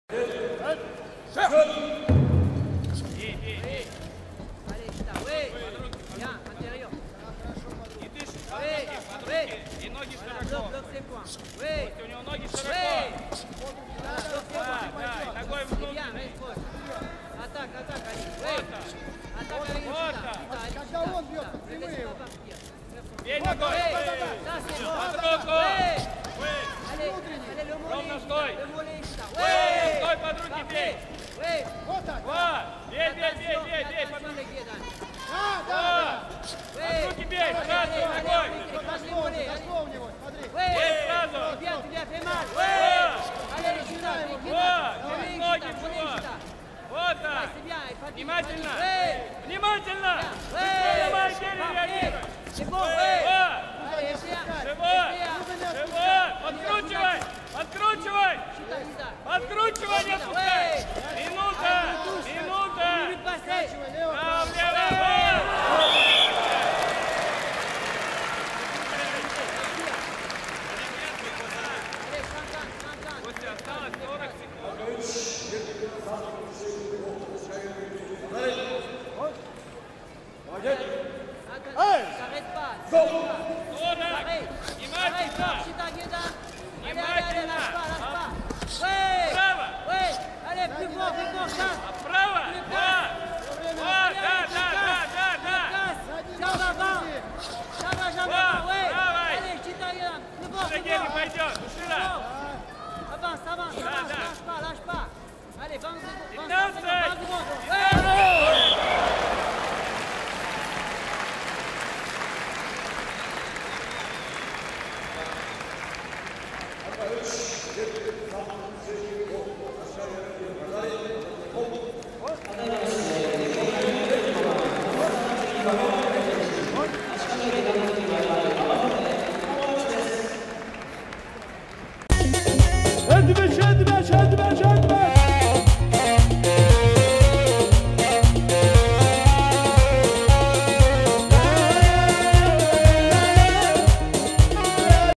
Konstantin Kovalenko (Russia) Vs Ali Zeghbab (France)/ippon
The_12th_World_Open_Karate_Tournament​ (iko1 Matsui )~ Japan 22-24 November 2019.